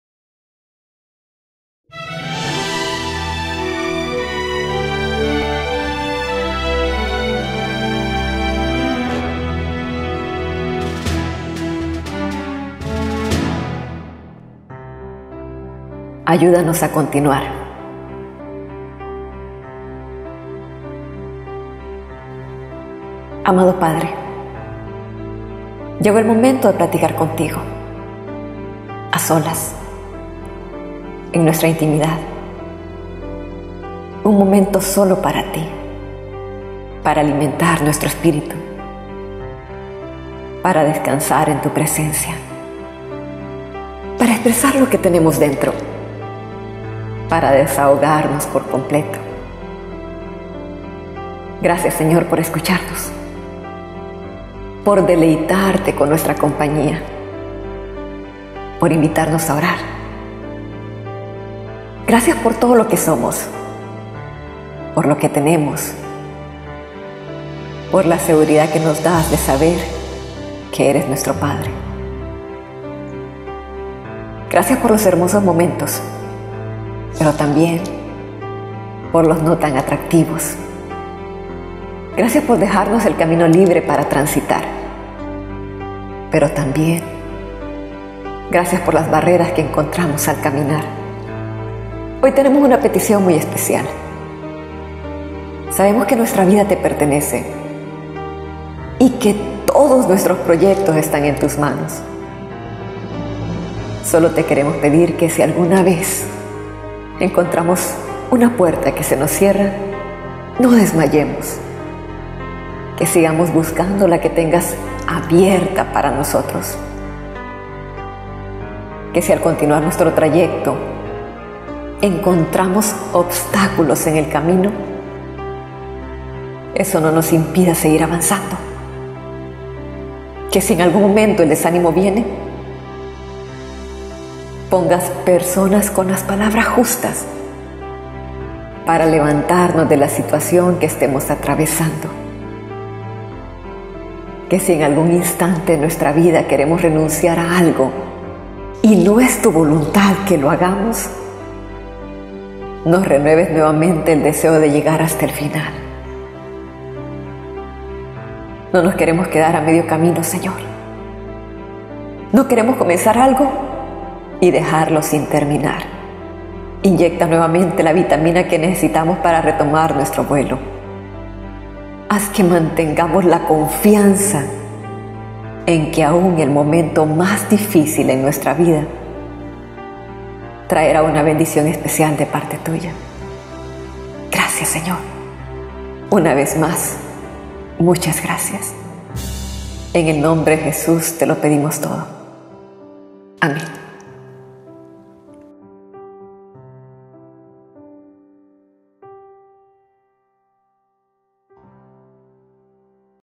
Oraciones en tiempo del Coronavirus... "AYUDANOS A CONTINUAR"
AYUDANOS A CONTINUAR
Amado Padre, llegó el momento de platicar contigo, a solas, en nuestra intimidad. Un momento solo para ti, para alimentar nuestro espíritu, para descansar en tu presencia, para expresar lo que tenemos dentro, para desahogarnos por completo.
Gracias Señor por escucharnos, por deleitarte con nuestra compañía, por invitarnos a orar, gracias por todo lo que somos, por lo que tenemos, por la seguridad que nos das de saber que eres nuestro Padre. Gracias por los hermosos momentos, pero también por los no tan atractivos, gracias por dejarnos el camino libre para transitar, pero también gracias por las barreras que encontramos al caminar.
Hoy tenemos una petición muy especial Señor, sabemos que nuestra vida te pertenece y que todos nuestros proyectos están en Tus manos, solo te queremos pedir que si alguna vez encontramos que una puerta se nos cierra, no desmayemos, que sigamos buscando la que tengas abierta para nosotros.
Que si al continuar nuestro trayecto, encontramos obstáculos en el camino, eso no nos impida seguir avanzando.
Que si en algún momento el desanimo viene, pongas personas con las palabras justas, para levantarnos de la situación que estemos atravesando,
Que si en algún instante de nuestra vida queremos renunciar a algo y NO es Tú voluntad que lo hagamos, nos renueves nuevamente el deseo de llegar hasta el final.
No nos queremos quedar a medio camino Señor, no queremos comenzar algo y dejarlo sin terminar, inyecta nuevamente la vitamina que necesitamos para retomar nuestro vuelo.
Has que mantengamos la confianza en que aún el momento más difícil en nuestra vida, traerá una bendición especial de parte tuya.
Gracias Señor, una vez más, muchas gracias.
Ene nombre de Jesús te lo pedimos todo.
Amén.